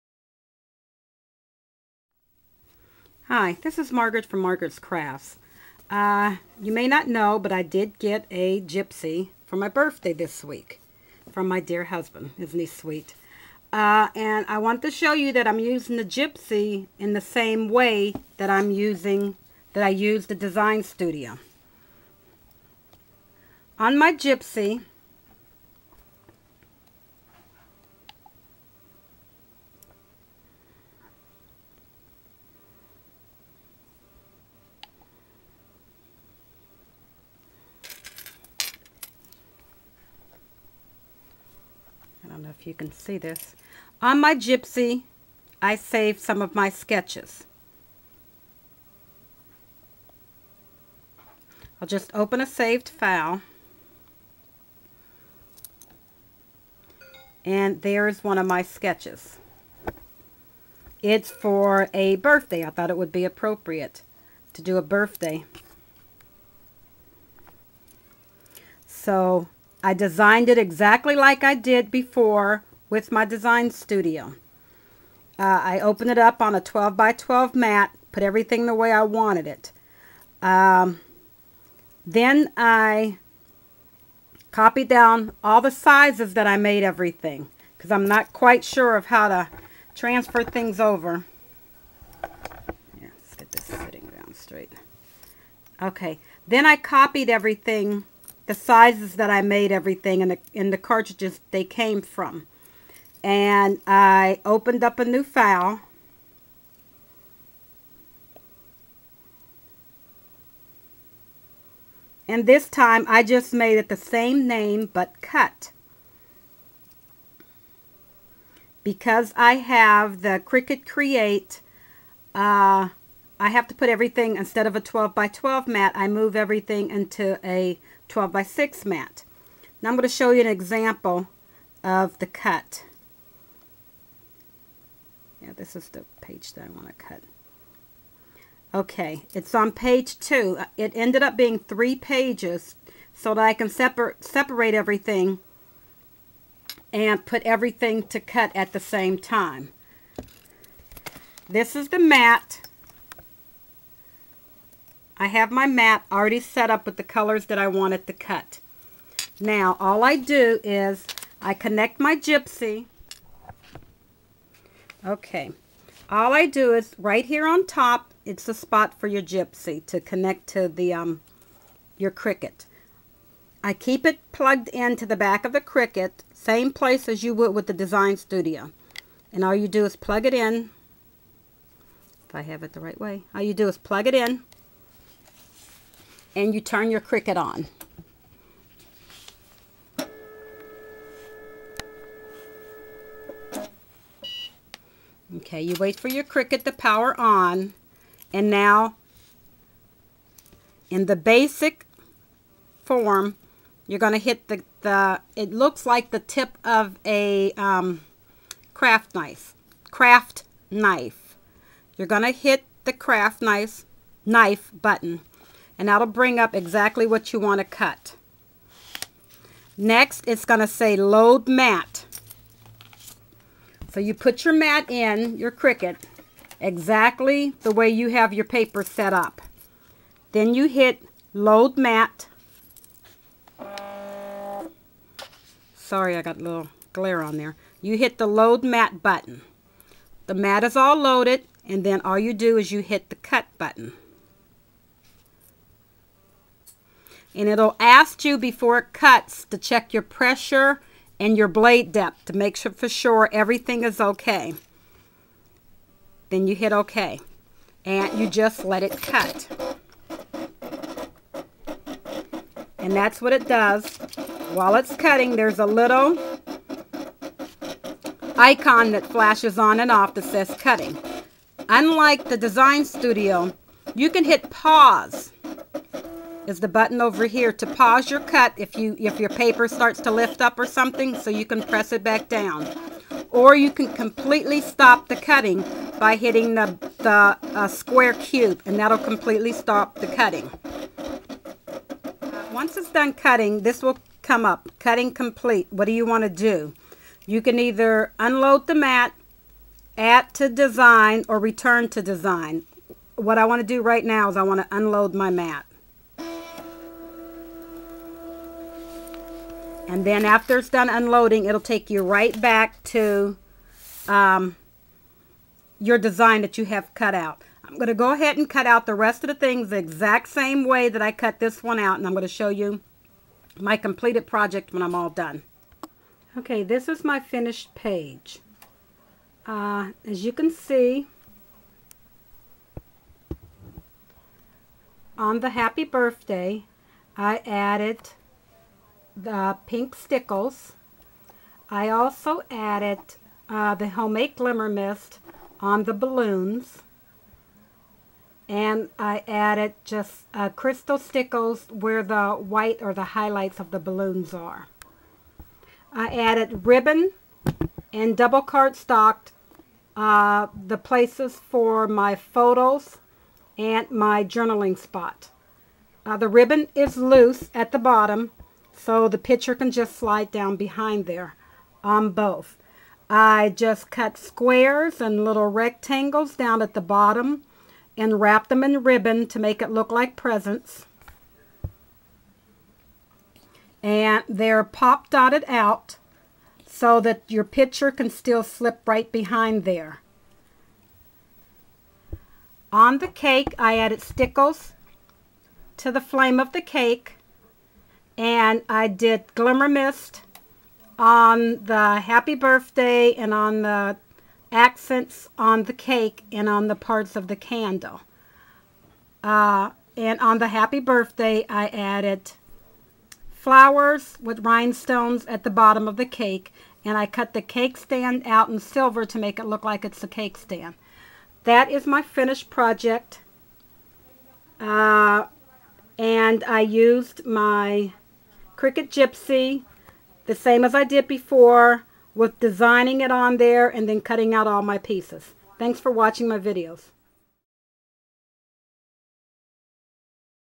Using the Gypsy to design and cut
This video shows how I use the cricut gypsy the same way that I use design studio.